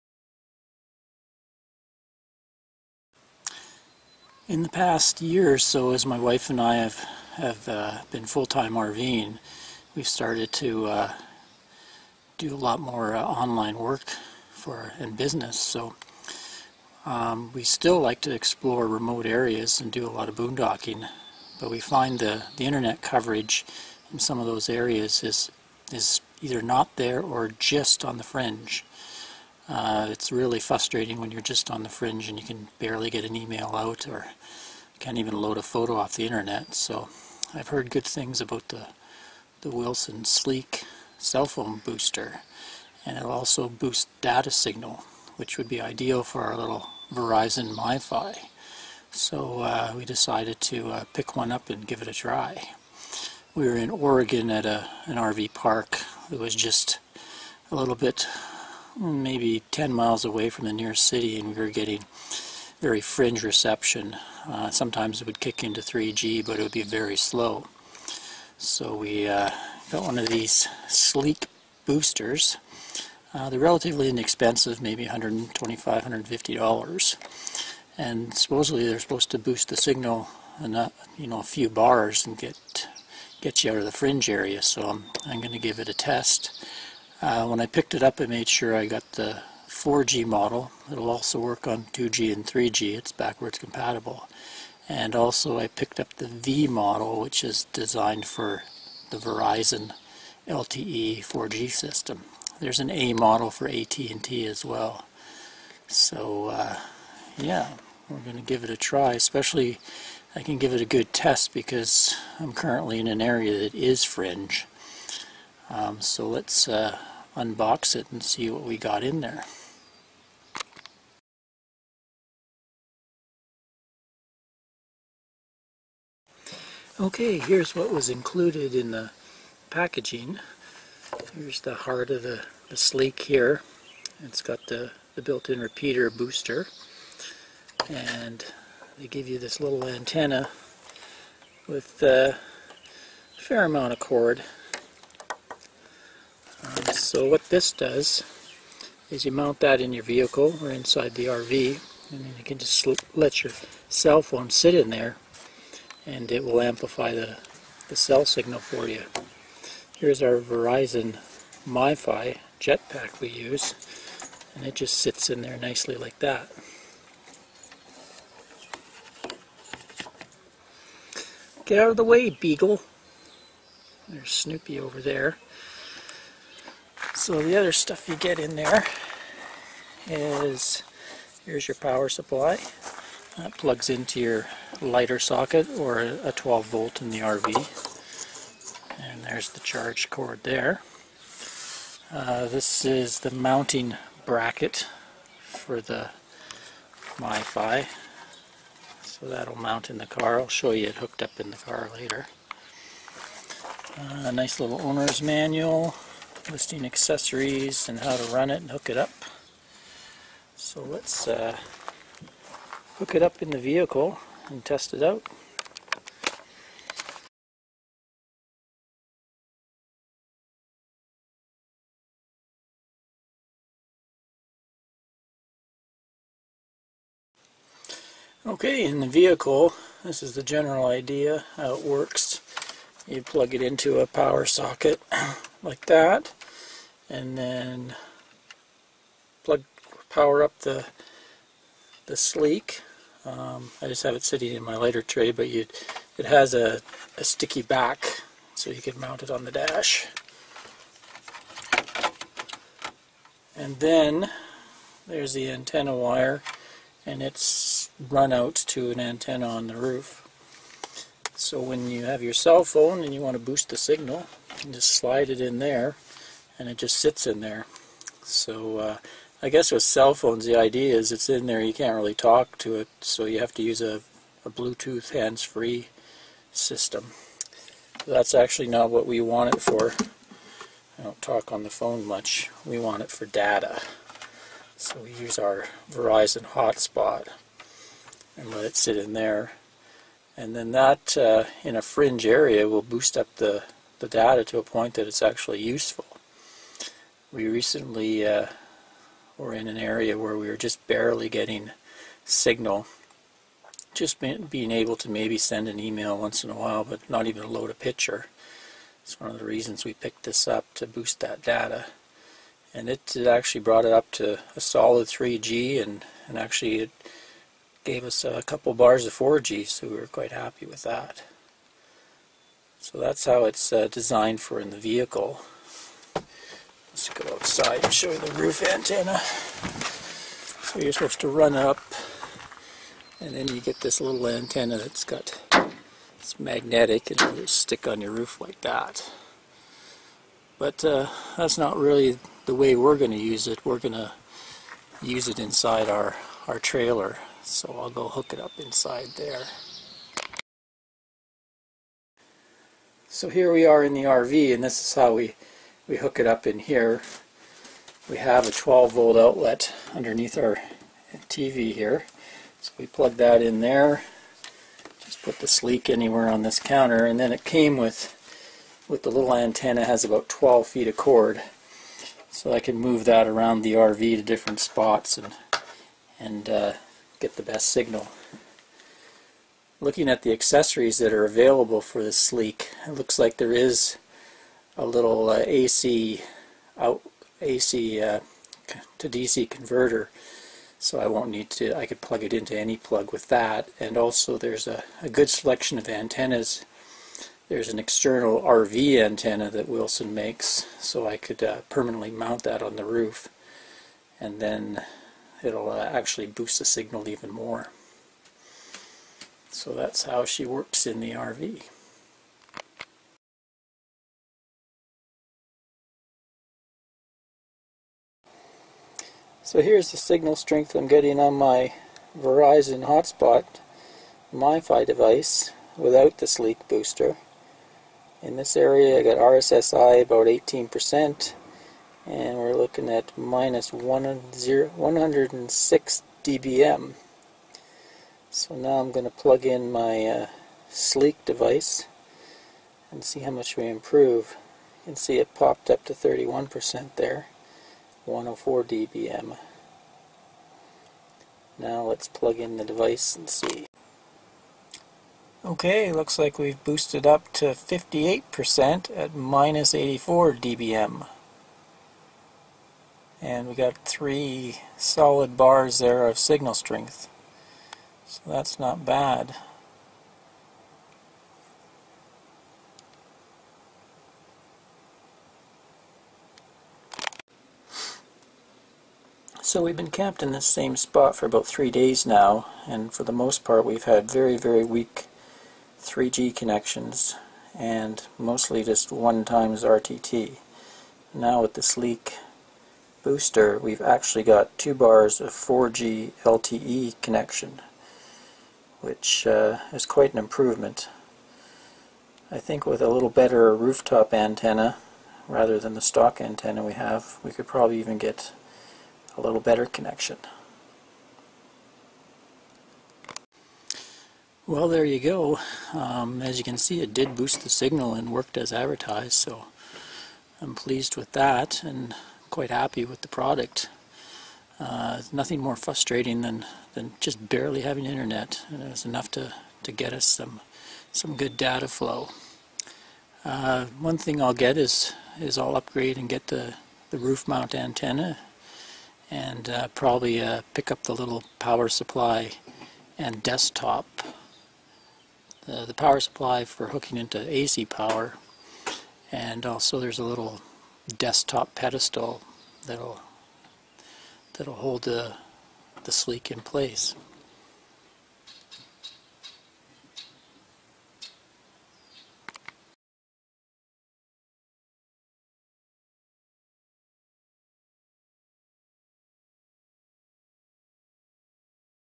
Boosting RV Internet with the Wilson Sleek 4G
In this video I unpack and demo the Wilson Sleek 4G in our truck and RV. I also show some real world examples of the cell signal boost we attained.

As we venture further afield (as we often do to boondock) the signal quality drops first to 3G and then to an extremely slow connection called 1xRTT. I'll spare you the technical mumbo jumbo and just say 1xRTT is painfully slow. Loading a web page can take minutes and uploading a photo usually times out. Because we are now really into exploring more remote areas and boondocking we needed to find a way to boost our incoming cell signal. So after I did some research online and talked to other veteran full-time RVers the most cost effective solution for our needs seemed to be a product made by Wilson Electronics called the Sleek 4G.